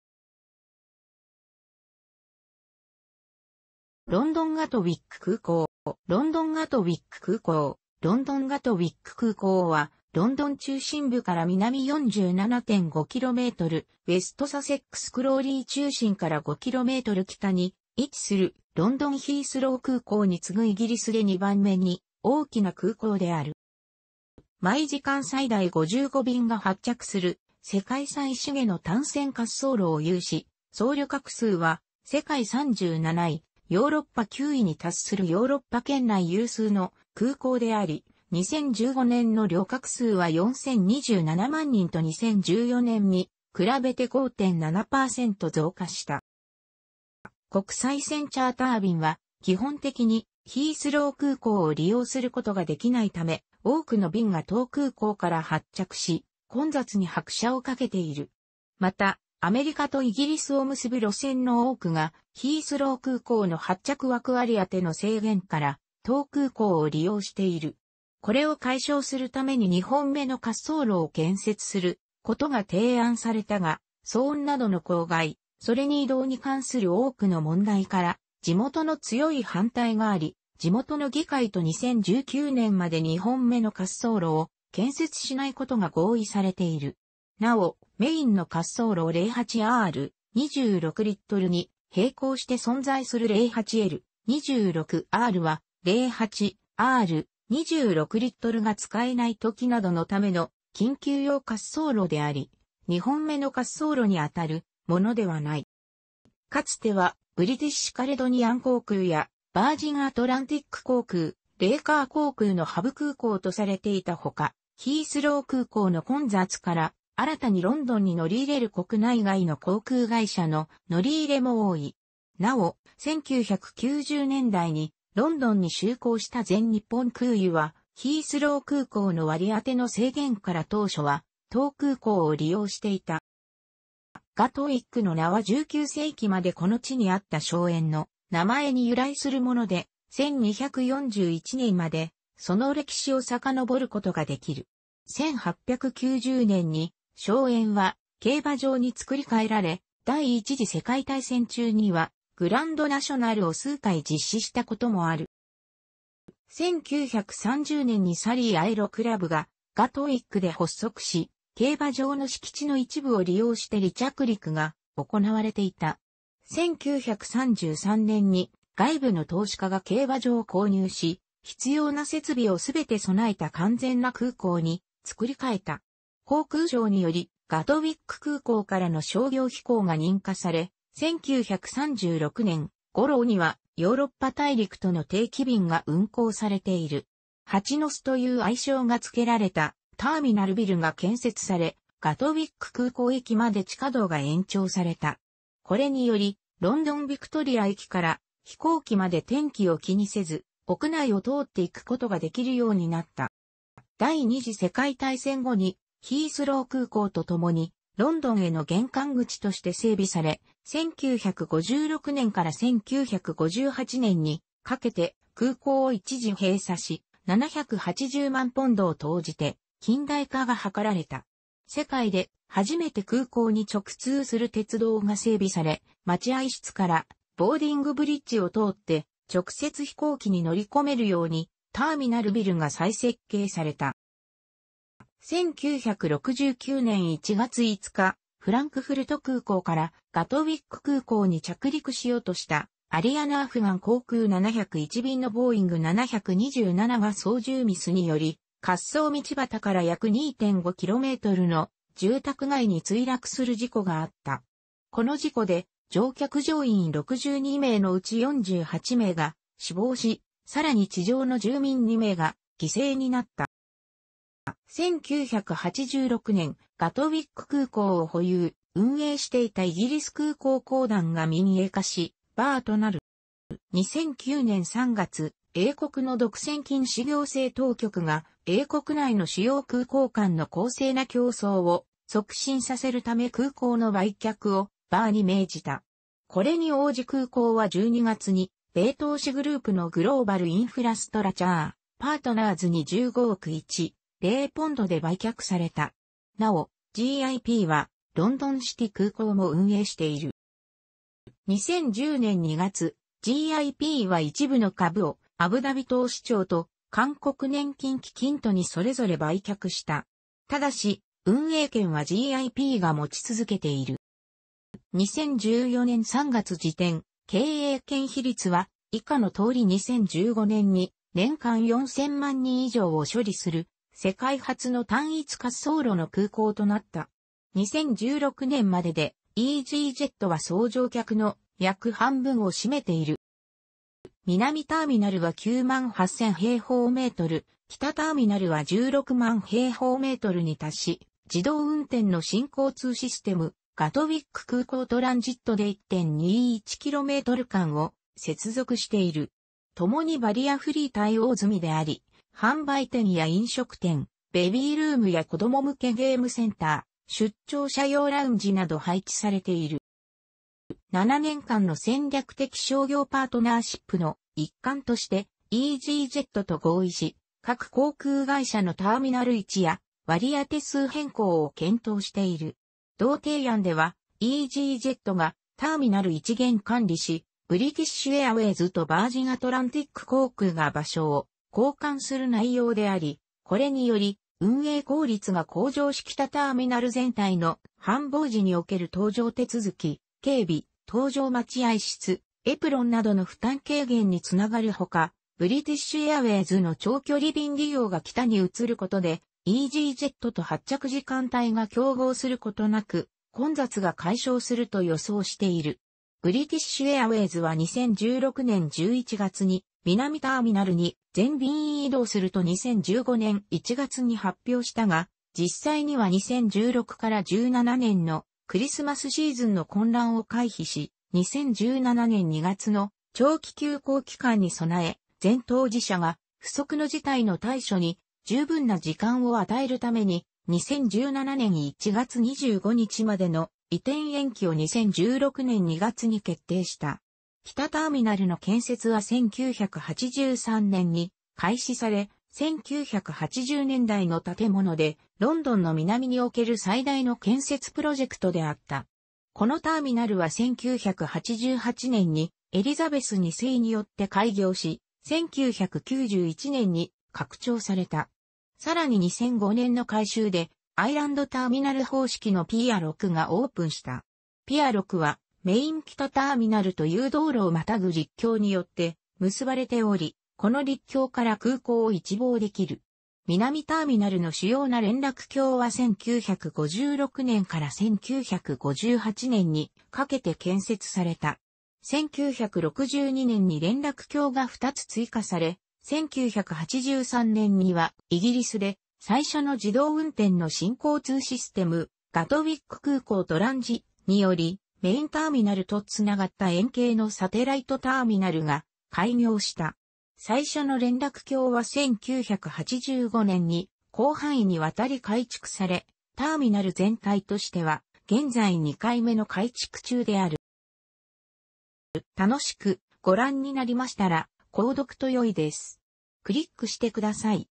ロンドン・ガトウィック空港
ロンドン・ガトウィック空港（ロンドン・ガトウィックくうこう）は、ロンドン中心部から南47.5km、ウェスト・サセックスクローリー中心から5km北に位置する、ロンドン・ヒースロー空港に次ぐイギリスで2番目に大きな空港である。

毎時間最大55便が発着する世界最繁の単線滑走路を有し、総旅客数は世界37位、ヨーロッパ9位に達するヨーロッパ圏内有数の空港であり、2015年の旅客数は4027万人と2014年に比べて5.7％増加した。

国際線チャーター便は基本的にヒースロー空港を利用することができないため、多くの便が当空港から発着し、混雑に拍車をかけている。また、アメリカとイギリスを結ぶ路線の多くが、ヒースロー空港の発着枠割り当ての制限から当空港を利用している。これを解消するために2本目の滑走路を建設することが提案されたが、騒音などの公害、それに移動に関する多くの問題から地元の強い反対があり、地元の議会と2019年まで2本目の滑走路を建設しないことが合意されている。なお、メインの滑走路08R/26Lに並行して存在する08L/26Rは、08R/26Lが使えないときなどのための緊急用滑走路であり、2本目の滑走路にあたるものではない。

かつてはブリティッシュ・カレドニアン航空やヴァージン・アトランティック航空、レイカー航空のハブ空港とされていたほか、ヒースロー空港の混雑から、新たにロンドンに乗り入れる国内外の航空会社の乗り入れも多い。なお、1990年代にロンドンに就航した全日本空輸は、ヒースロー空港の割り当ての制限から当初は当空港を利用していた。

ガトウィックの名は19世紀までこの地にあった荘園の名前に由来するもので、1241年までその歴史を遡ることができる。1890年に荘園は競馬場に作り変えられ、第一次世界大戦中にはグランドナショナルを数回実施したこともある。

1930年にサリー・アエロ・クラブ (Surrey Aero Club) がガトウィックで発足し、競馬場の敷地の一部を利用して離着陸が行われていた。1933年に外部の投資家が競馬場を購入し、必要な設備を全て備えた完全な空港に作り変えた。航空相によりガトウィック空港からの商業飛行が認可され、1936年ごろにはヨーロッパ大陸との定期便が運航されている。「蜂の巣」という愛称がつけられたターミナルビルが建設され、ガトウィック空港駅まで地下道が延長された。これによりロンドン・ヴィクトリア駅から飛行機まで天気を気にせず屋内を通って行くことができるようになった。

第二次世界大戦後にヒースロー空港とともにロンドンへの玄関口として整備され、1956年から1958年にかけて空港を一時閉鎖し、780万ポンドを投じて近代化が図られた。世界で初めて空港に直通する鉄道が整備され、待合室からボーディング・ブリッジを通って直接飛行機に乗り込めるようにターミナルビルが再設計された。

1969年1月5日、フランクフルト空港からガトウィック空港に着陸しようとしたアリアナ・アフガン航空701便のボーイング727が操縦ミスにより滑走路端から約2.5 kmの住宅街に墜落する事故があった。この事故で乗客乗員62名のうち48名が死亡し、さらに地上の住民2名が犠牲になった。

1986年、ガトウィック空港を保有・運営していたイギリス空港公団が民営化し、BAAとなる。

2009年3月、英国の独占禁止行政当局が、英国内の主要空港間の公正な競争を促進させるため空港の売却をBAAに命じた。これに応じ空港は12月に米投資グループのグローバル・インフラストラクチャー・パートナーズ (GIP) に15億1,000万ポンドで売却された。なお、GIPはロンドン・シティ空港も運営している。

2010年2月、GIPは一部の株をアブダビ投資庁と韓国年金基金とにそれぞれ売却した。ただし、運営権はGIPが持ち続けている。

2014年3月時点、経営権比率は以下の通り
2015年に、年間4000万人以上を処理する世界初の単一滑走路の空港となった。2016年までで、イージージェットは総乗客の約半分を占めている。

南ターミナルは98000平方メートル、北ターミナルは160,000平方メートルに達し、自動運転の新交通システム「ガトウィック空港トランジット」で1.21km間を接続している。
共にバリアフリー対応済みであり、販売店や飲食店、ベビールームや子供向けゲームセンター、出張者用ラウンジなど配置されている。